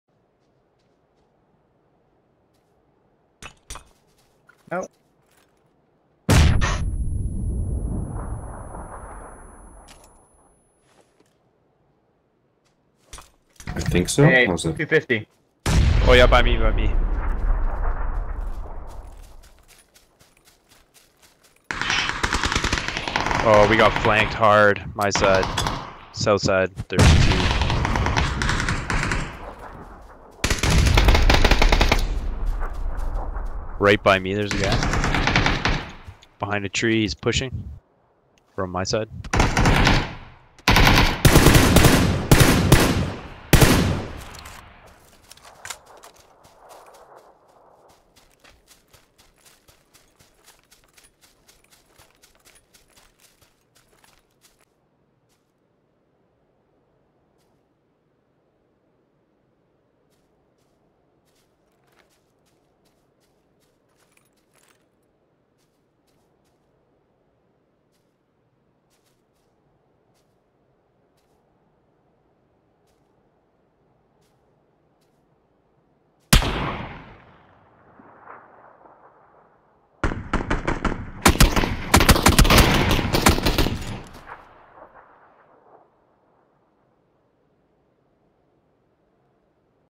Suicide team
With position and gear my team decided to rush out of circle and force a fight.